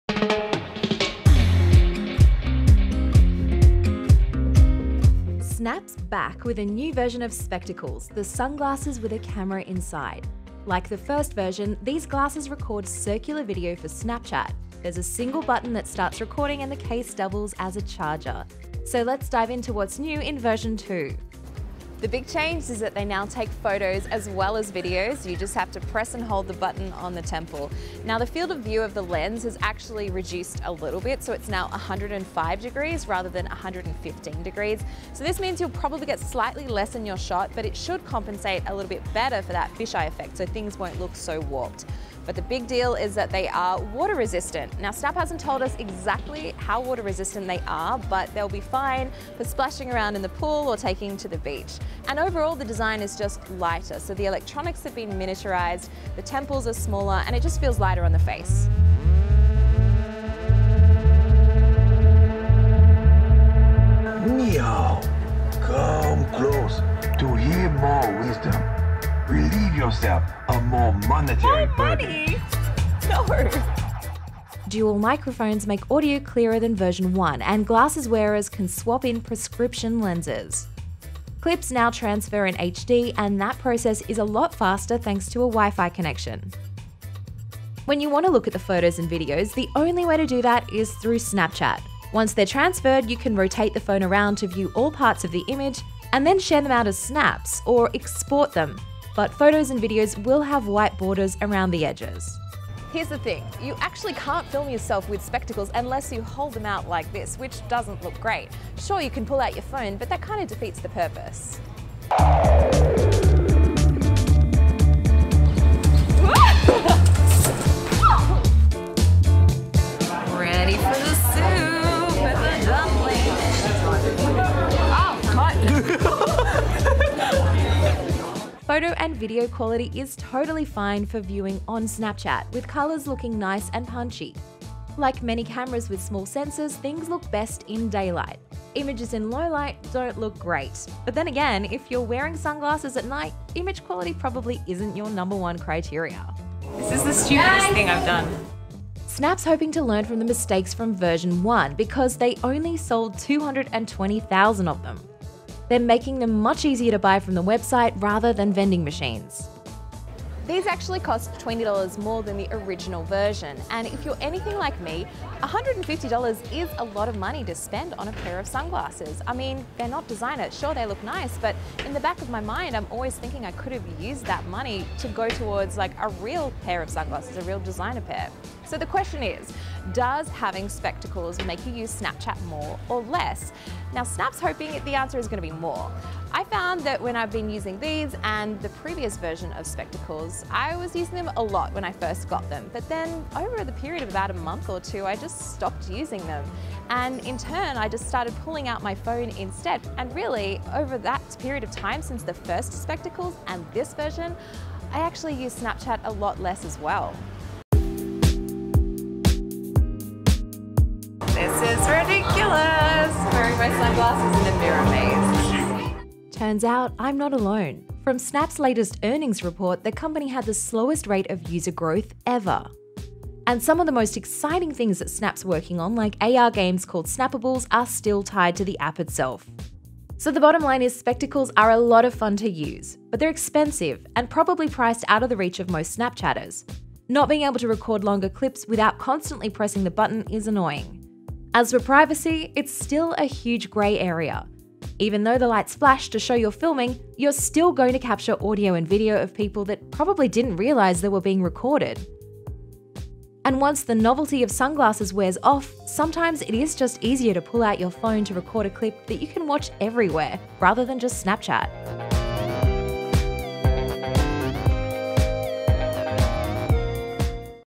Snapchat Spectacles review (2018)
At $150, these sunglasses with a camera are pretty pricey.